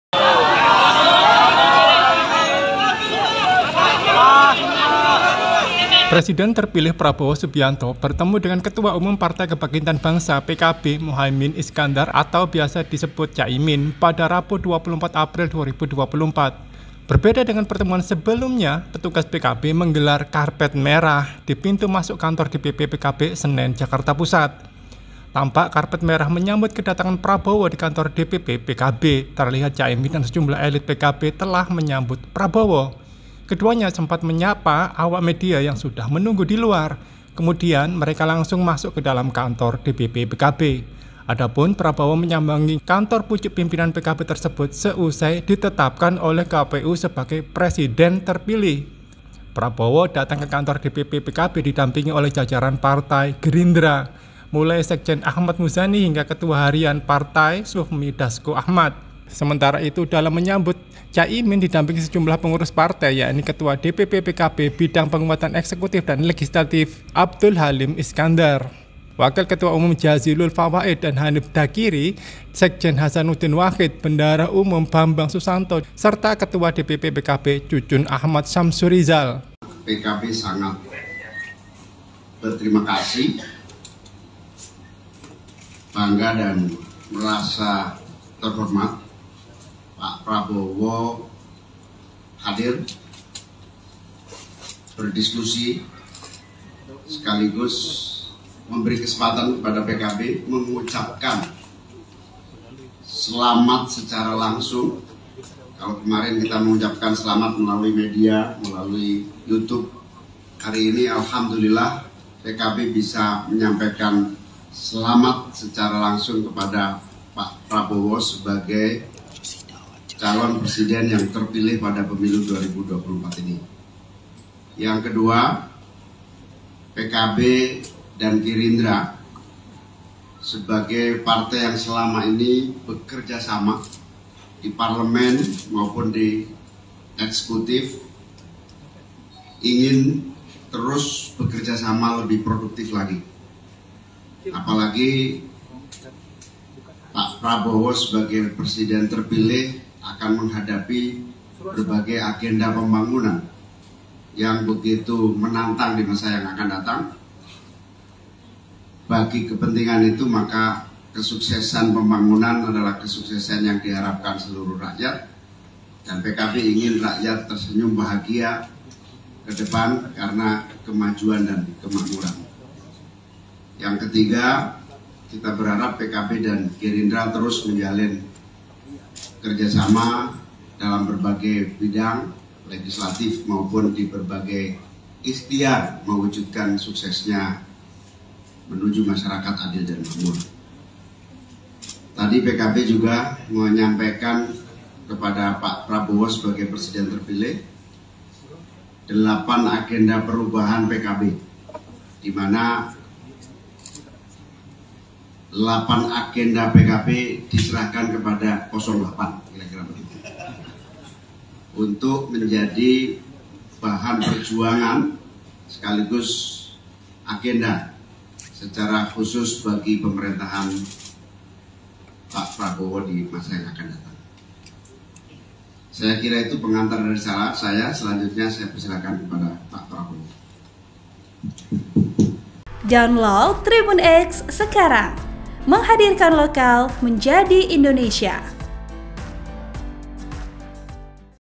BEGINI ISI PERTEMUANNYA, Cak Imin Gelar Karpet Merah Sambut Kedatangan Prabowo
[ tw ]

SURYA.CO.ID - Cak Imin menggelar karpet merah untuk Presiden Terpilih Prabowo Subainto yang hadir di kantor DPP PKB pada Rabu (24/4).
Seusai pertemuan tersebut, Anies mengatakan PKB dan Gerindra menginginkan kerja sama lebih produktif.

Cak Imin berharap kedua partai itu bisa bekerja sama di berbagai bidang.
Ia pun menyerahkan 8 agenda perubahan PKB kepada Prabowo.